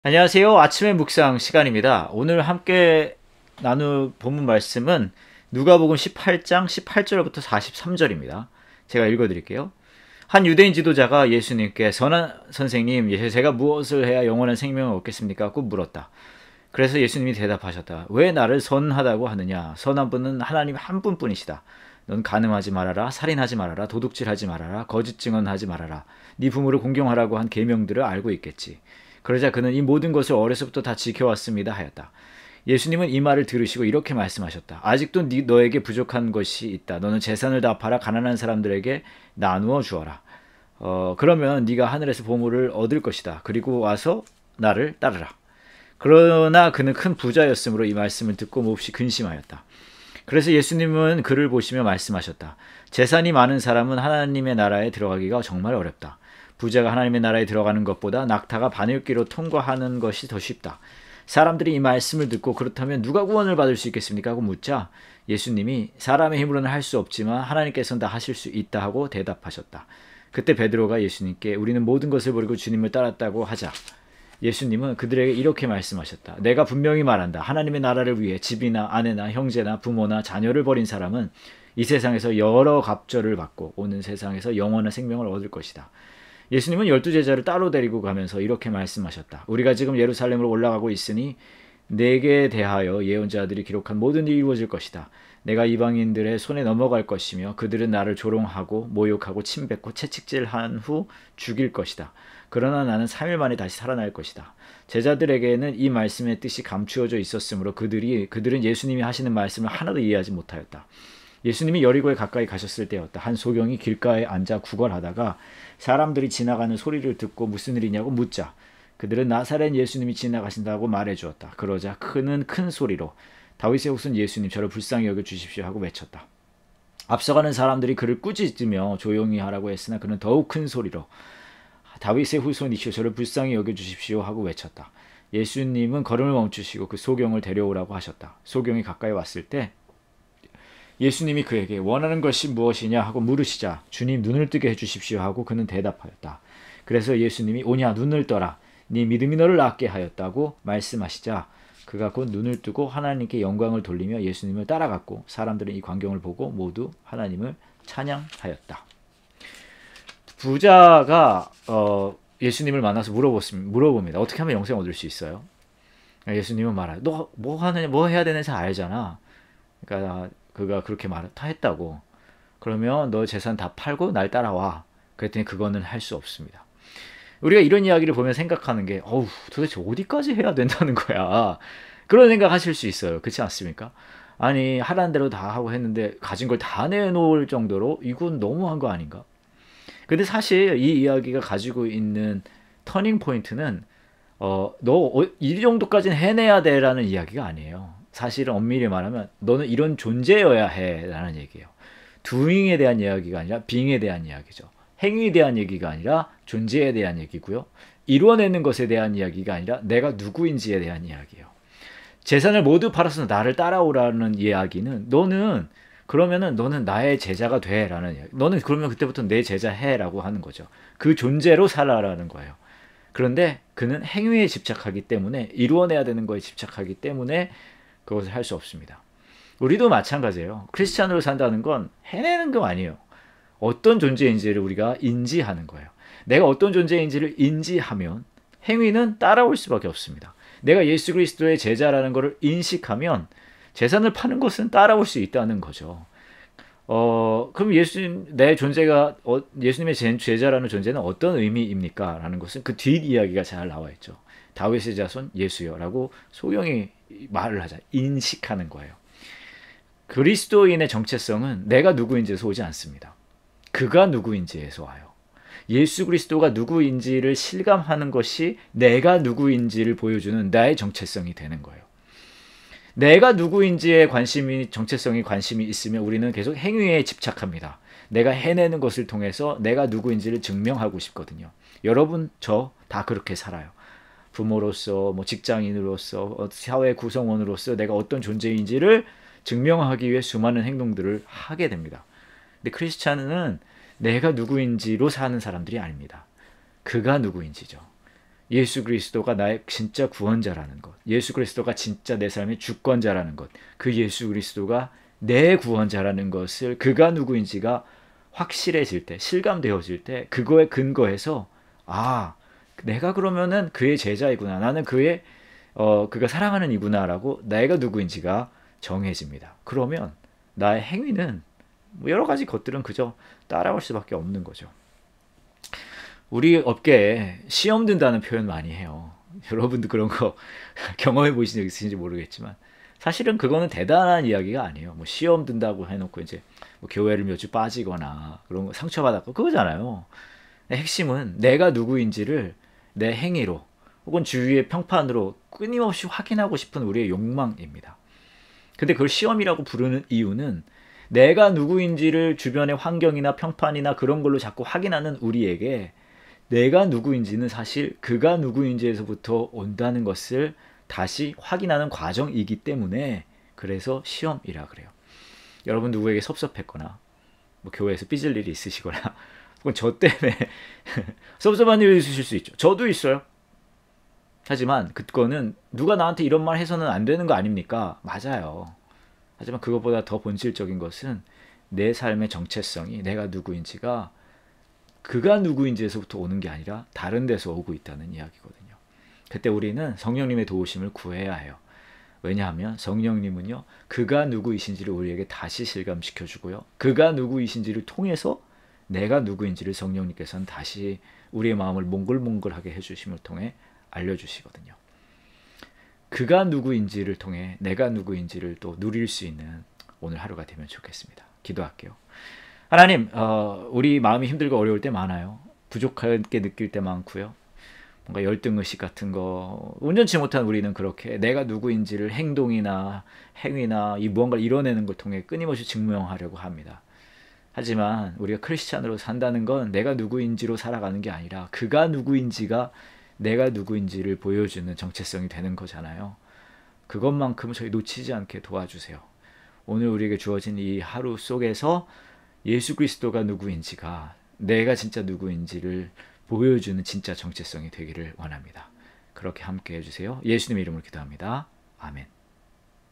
12/13(수) [아침N묵상] | 와플터치QT | 누가복음 18:18-43(써어니)
18:18 한 유대인 지도자가 예수님께 '선한 선생님. 제가 무엇을 해야 영원한 생명을 얻겠습니까 ?' 하고 물었다.

18:19 그래서 예수님이 대답하셨다. '왜 나를 선하다고 하느냐 ? 선한 분은 하나님 한 분뿐이시다.

18:20 너는 '간음하지 말아라. 살인하지 말아라. 도둑질하지 말아라. 거짓 증언하지 말아라. 네 부모를 공경하라.'고 한 계명들을 알고 있겠지'

18:21 그러자 그는 '이 모든 것을 어려서부터 다 지켜왔습니다.' 하였다.

18:22 예수님은 이 말을 들으시고 이렇게 말씀하셨다. '아직도 네게 한 가지 부족한 것이 있다. 너는 재산을 다 팔아 가난한 사람들에게 나눠주어라. 그러면 네가 하늘에서 보물을 얻을 것이다. 그리고 와서 나를 따르라.'

18:23 그러나 그는 큰 부자였으므로 이 말씀을 듣고 몹시 근심하였다.

18:24 그래서 예수님은 그를 보시며 말씀하셨다. '재산이 많은 사람은 하나님의 나라에 들어가기가 정말 어렵다.

18:25 부자가 하나님의 나라에 들어가는 것보다 낙타가 바늘귀로 통과하는 것이 더 쉽다.'

18:26 사람들이 이 말씀을 듣고 '그렇다면 누가 구원받을 수 있겠습니까 ?'하고 묻자

18:27 예수님이 '사람의 힘으로는 할 수 없지만 하나님께서는 다 하실 수 있다.' 하고 대답하셨다.

18:28 그때 베드로가 예수님께 '우리는 모든 것을 버리고 주님을 따랐습니다.' 하자

18:29 예수님은 그들에게 이렇게 말씀하셨다. '내가 분명히 말한다. 하나님의 나라를 위해 집이나 아내나 형제나 부모나 자녀를 버린 사람은

18:30 이 세상에서 여러 갑절로 받고, 오는 세상에서는 영원한 생명을 얻을 것이다.'

18:31 예수님은 열 두 제자를 따로 데리고 가시면서 이렇게 말씀하셨다. '우리가 지금 예루살렘으로 올라가고 있으니 내게 대하여 예언자들이 기록한 모든 일이 이루어질 것이다.

18:32 내가 이방인들의 손에 넘어갈 것이며 그들은 나를 조롱하고 모욕하고 침 뱉고

18:33 채찍질한 후 죽일 것이다. 그러나 나는 3일 만에 다시 살아날 것이다.'

18:34 제자들에게는 이 말씀의 뜻이 감추어져 있었으므로 그들은 예수님이 하시는 말씀을 하나도 이해하지 못하였다.

18:35 예수님이 여리고에 가까이 가셨을 때였다. 한 소경이 길가에 앉아 구걸하다가

18:36 사람들이 지나가는 소리를 듣고 무슨 일이냐고 묻자

18:37 그들은 '나사렛 예수님이 지나가신다.' 하고 말해 주었다.

18:38 그러자 그는 큰 소리로 '다윗의 후손 예수님, 저를 불쌍히 여겨 주십시오.' 하고 외쳤다.

18:39 앞서 가는 사람들이 그를 꾸짖으며 조용히 하라고 했으나 그는 더욱 큰 소리로 '다윗의 후손이시여, 저를 불쌍히 여겨 주십시오.' 하고 외쳤다.

18:40 예수님은 걸음을 멈추시고 그 소경을 데려오라고 하셨다. 소경이 가까이 왔을 때

18:41 예수님이 그에게 '원하는 것이 무엇이냐 ?' 하고 물으시자 '주님, 눈을 뜨게 해 주십시오.' 하고 그는 대답하였다.

18:42 그래서 예수님이 '오냐, 눈을 떠라. 네 믿음이 너를 낫게 하였다.' 하고 말씀하시자

18:43 그가 곧 눈을 뜨고 하나님께 영광을 돌리며 예수님을 따라갔고 사람들은 이 광경을 보고 모두 하나님을 찬양하였다.